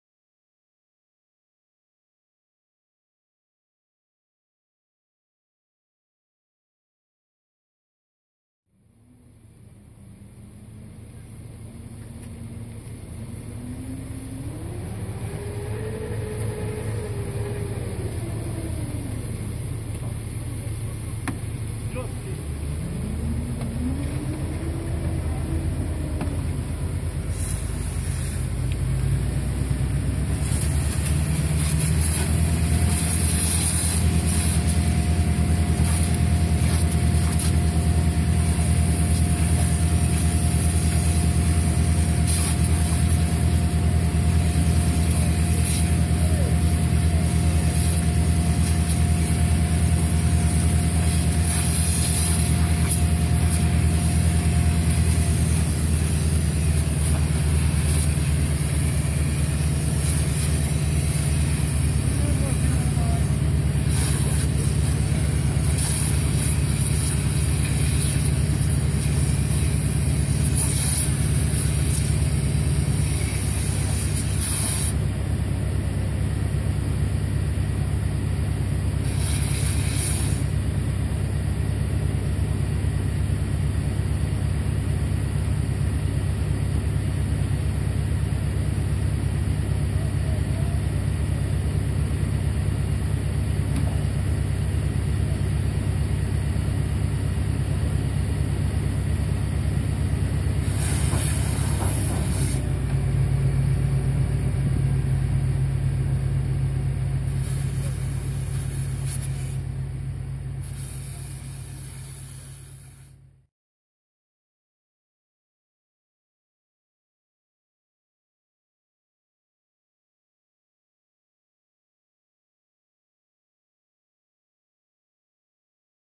MGS - Vehicle possibility - Extinguishing car in flame
New firefighing vehicle delivery by MGS Grupa Croatia.
Presentation and education of end user on possibilities and functionality of vehicle.
In this video was shown usage of end foam mixer for hp nozzle MGS STRONG with high pressure.

Vehicle with 5.000l of water, 300l of foam, combined pump 2000l/min @ 10bar and 400l/min @ 40 bar.
First intervention equipment, and system of front nozzles for road cleaning.